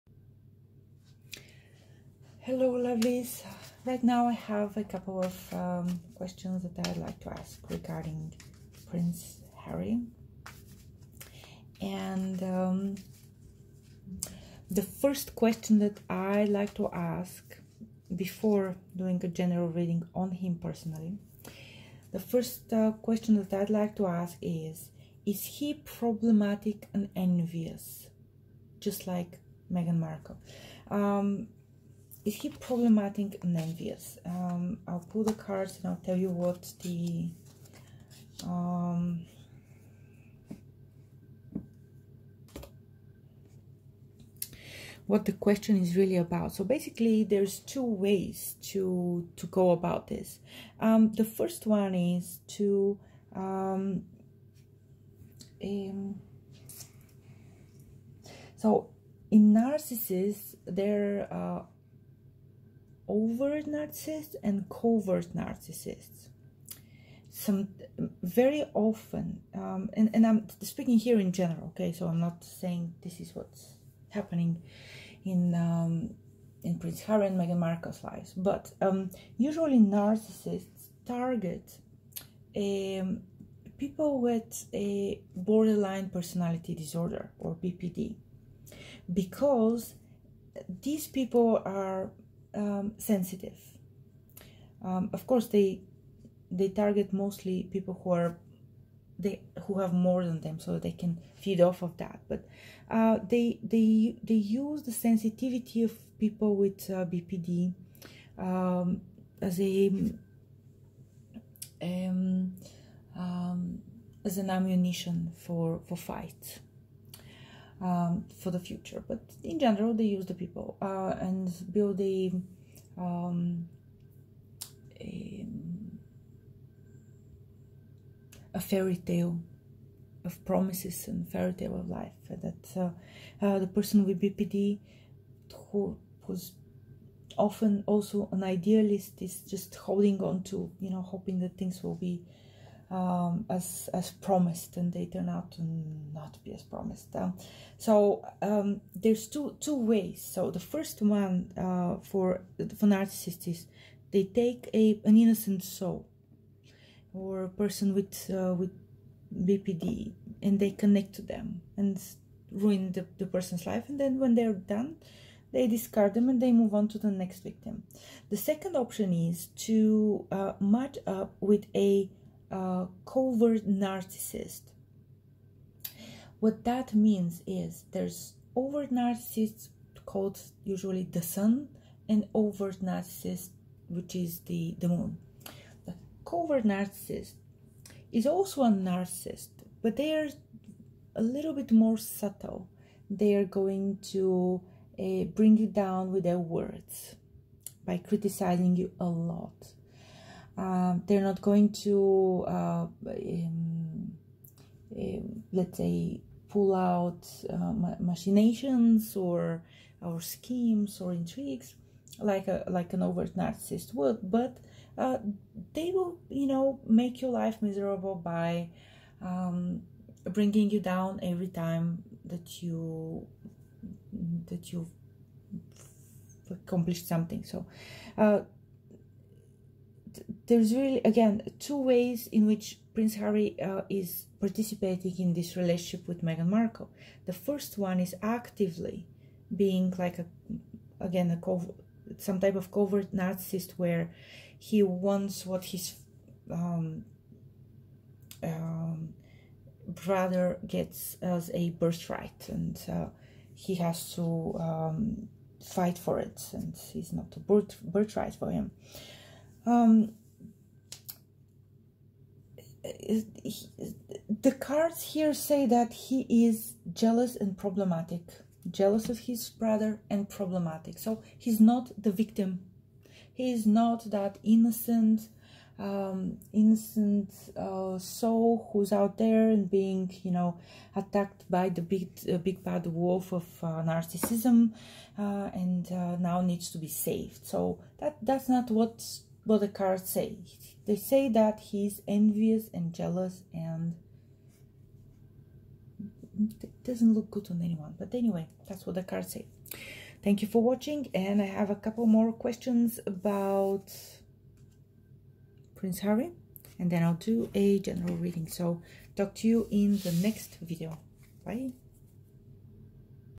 Is Prince Harry problematic and envious? Tarot spread
Today I did a general reading on Prince Harry and before I started the reading I asked the question: If prince Harry problematic and envious of Prince Willian?

I was expecting a no but that's not what I got. Interesting that the cards and the akasha show what's really going on, isn't it?

TTYS,
Arielle